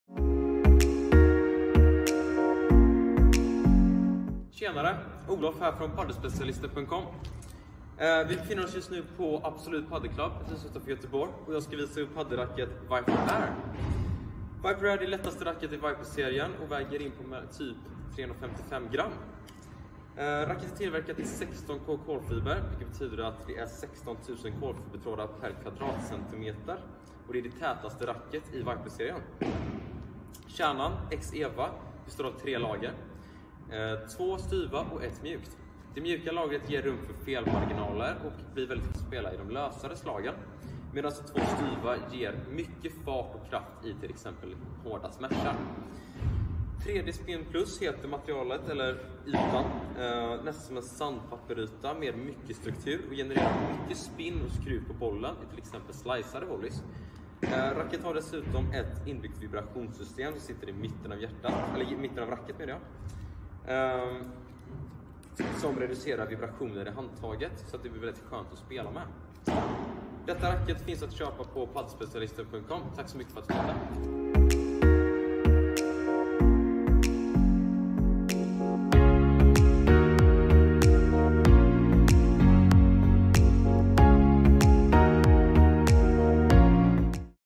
Padelspecialisten | Babolat Viper Air Padelracket
Babolat Viper Air

Babolat Viper Air är utvecklad för smidiga spelare som kräver ett racket som följer deras kvicka rörelser. Denna modell från Babolat ger bollen en explosiv fart med extrem styrförmåga för ett snabbt och offensivt spel.

Viper Air har en kärna uppbyggd av Babolats nyutvecklade X-EVA Technology. Detta innebär tre olika lager av EVA ger tillsammans en perfekt effekt. De två styva yttre lagren ger extra fart vid hårda slag medan ett mjukare inre lager ger ökad felmarginal och komfort på lösare slag.

100% karbon-träffyta bestående av 16K Carbon, perfekt anpassad för rackets form för att leverera explosivitet och en överraskande kontrollerad touch.
Den räfflade ytan kombinerat med en sandpapper-yta, 3D Spin+, ger maximalt grepp och spinn på bollen så att spelaren kan sätta ytterligare press på motståndaren.

Med nya Vibrasorb System får varje slag en utökad komfort. Detta vibrationsdämpande system är integrerat i racket hals ovanför handtaget och reducerar vibrationerna som uppstår i varje slag. Resultatet är en utökad komfort och en minskad risk för skador såsom tennisarmbåge.

Likt tidigare modeller från Babolat är Viper Air designad med HPS-Technology. Hole Pattern-System ger mer fart och kontroll tack vare att de borrade hålen i racket är anpassade efter rackets form och sweetspot. Detta ger mer effekt i slagen för mer fart och mer grepp på bollen för utökad spinn.